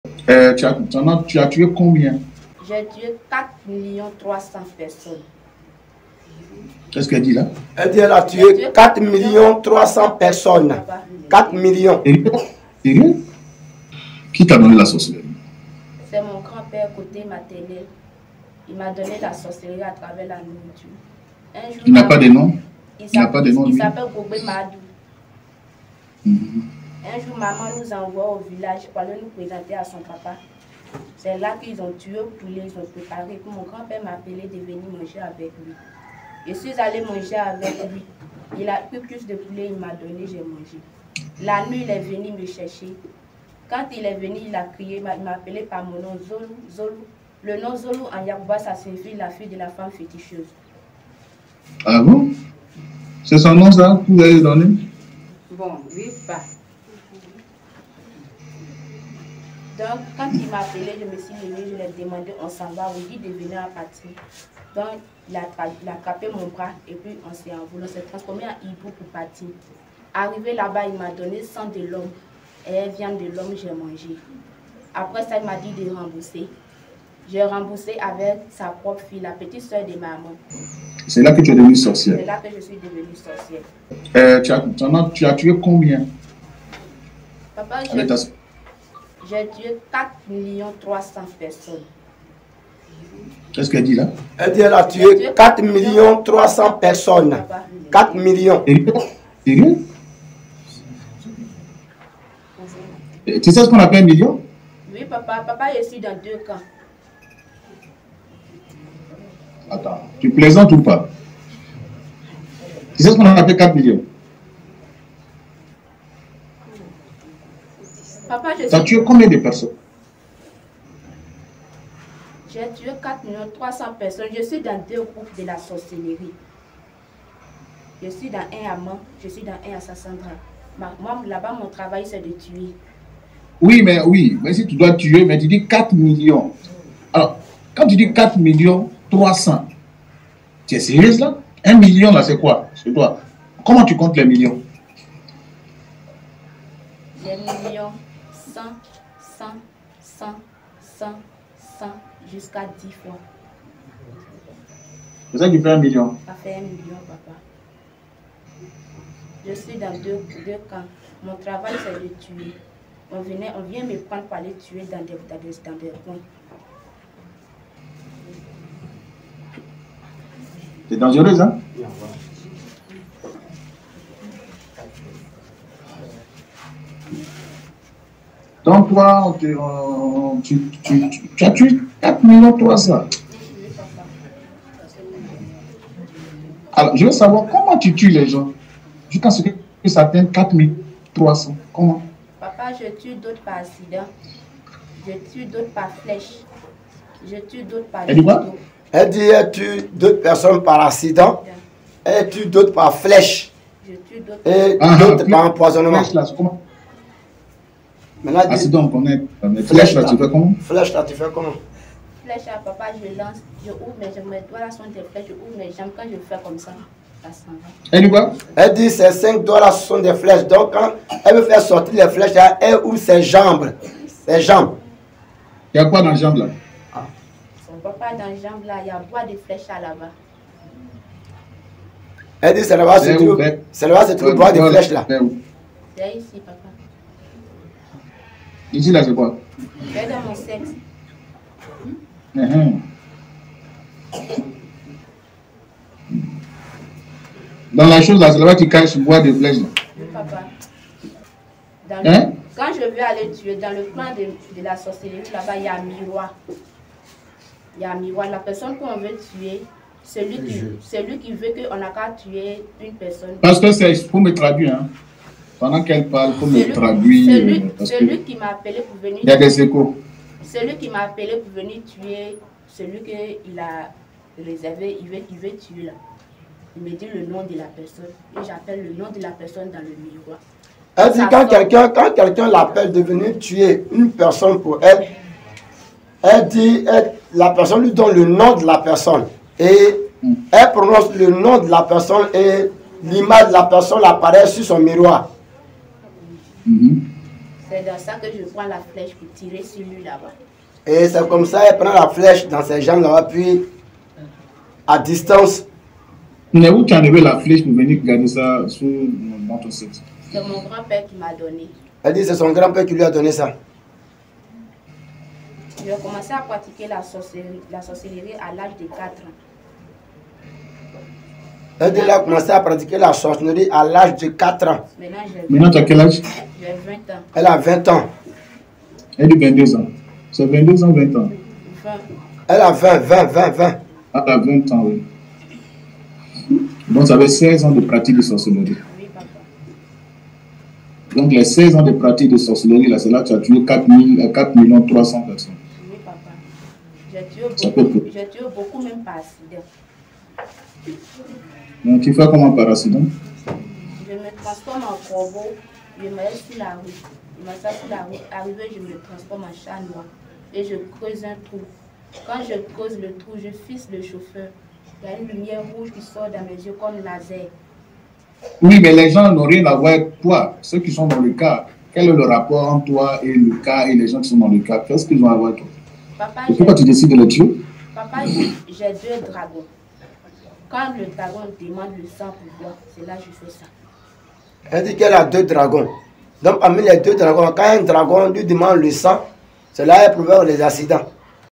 🔴16 ans de pratique de la sorcellerie, et plus de 4 millions 300 âmes sacrifiées à la sorcellerie
🔴Les révélations glaçantes d'une sorcière : " 16 ans de pratique de la sorcellerie, et plus de 4 millions 300 âmes sacrifiées à la sorcellerie."